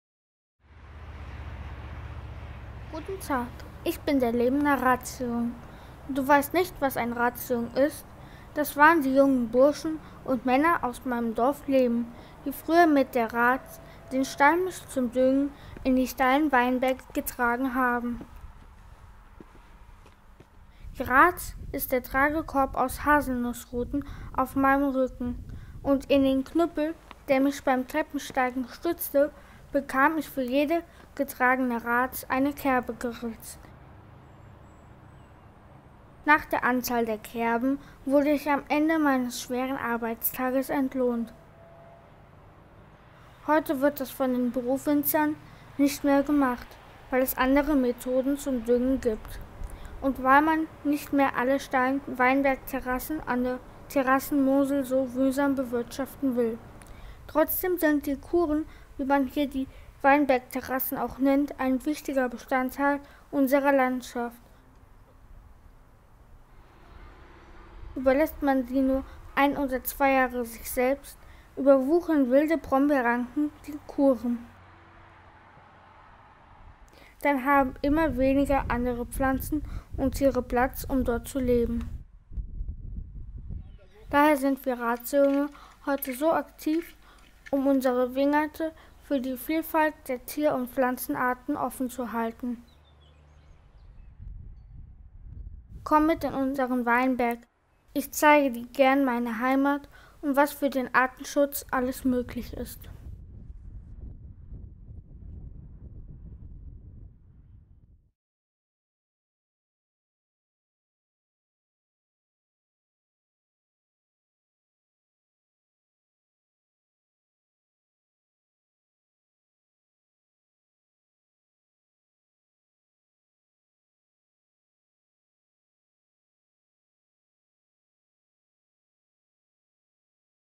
Der Lehmer Razejung
Der Lehmer Razejung lädt zum Naturerlebnis in der Würzlay ein!